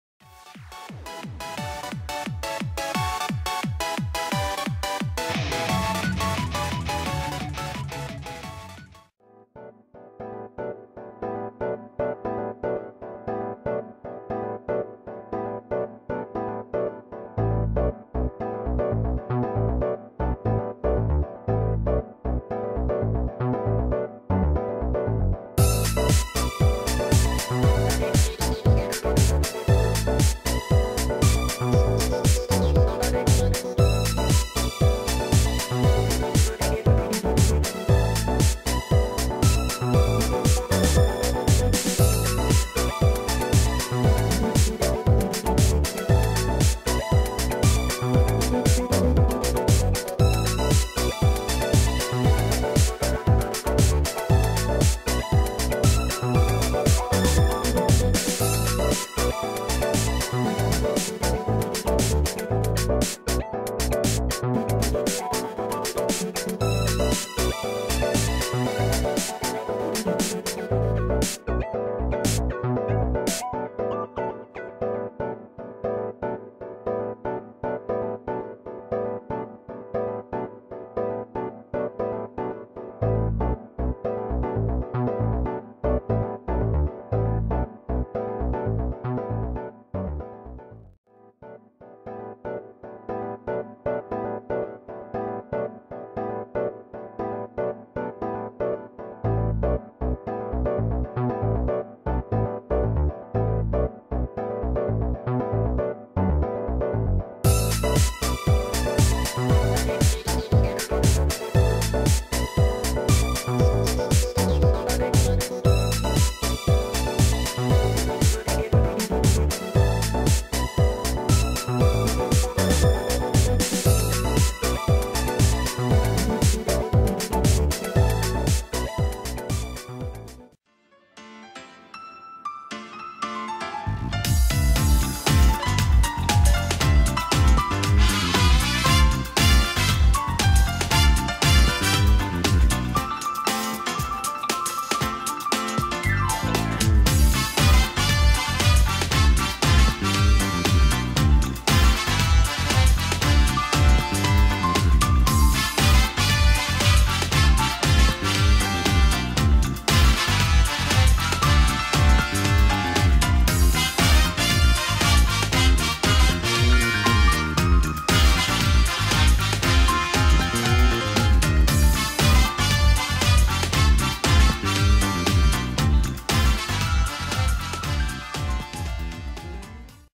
FlyingDream's General Parameter Calculator Update - RPG Maker MV
This is a calculator made for the Base Parameter Control and Class Base Parameters plugins! FlyingDream made this helpful tool to calculate the changes made to the various RPG Maker MV parameters and show their growth across different levels! This update now includes extended functionality for the Extra Parameter Formulas and Special Parameter Formulas plugins.